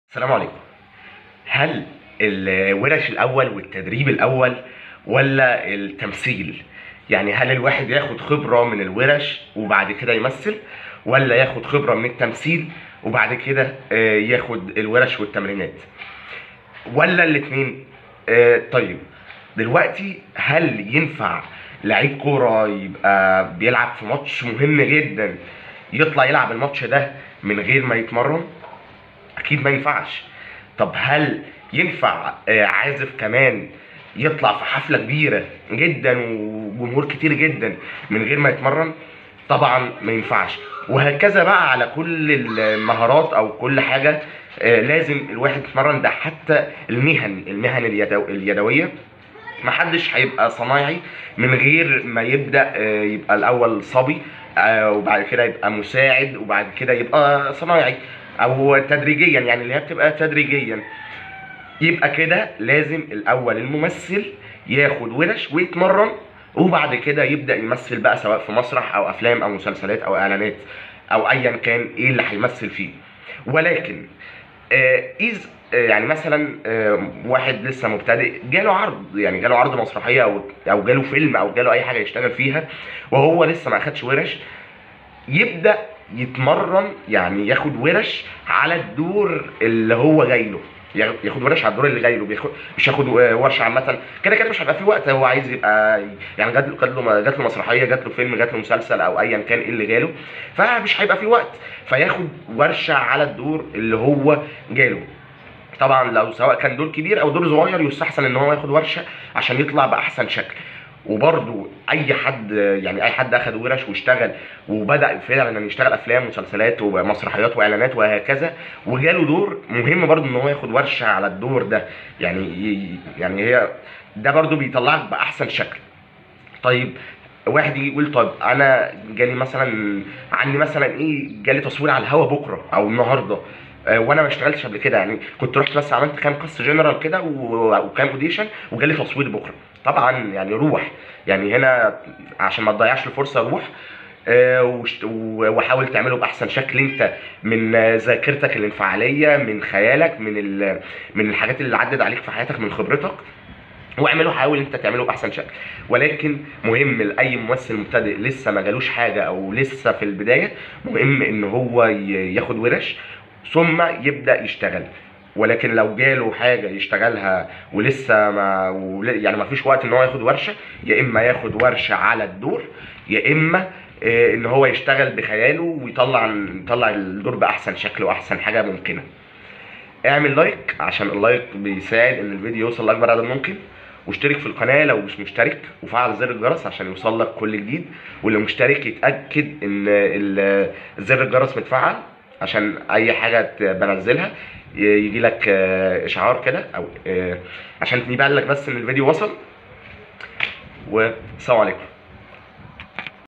الخبره من التمثيل الاول ولا الورش
هل البدايه من خلال التمثيل ام من خلال تمارين اعداد الممثل ( الورش )